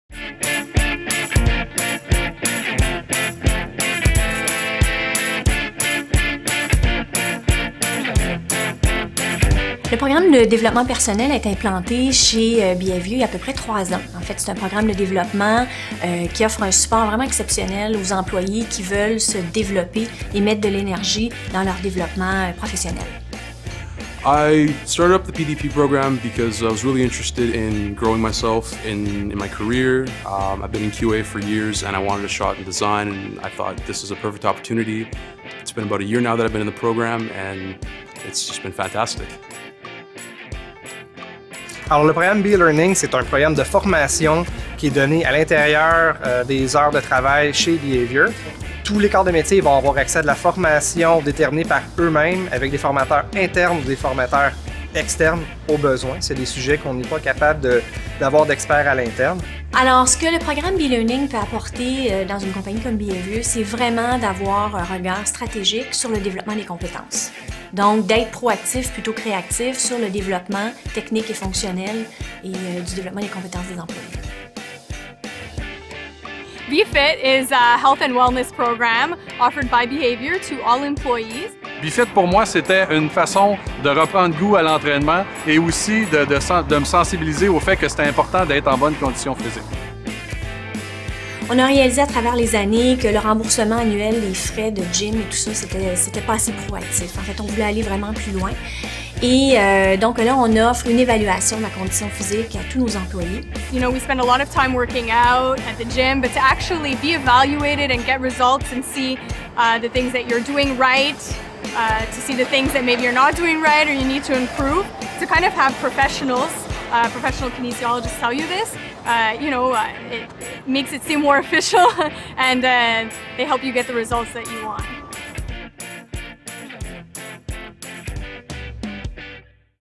Behaviour's employee programs / Les programmes de Behaviour pour les employés
Check out the programs implemented by Behaviour! Through BeFit, employees can have their fitness levels evaluated and a training plan created to help reach their fitness goals, while programs like Be Learning and the talent management program offer training and personal development which enable employees to expand their competencies.

Regardez les programmes mis en place par Behaviour! Grâce à BeFit, nos employés peuvent faire évaluer leur condition physique et ont accès à des plans de mise en forme personnalisés tandis que Be Learning et le programme de gestion du talent sont des programmes de formation et de développement personnel leur permettant d'élargir leurs champs de compétences.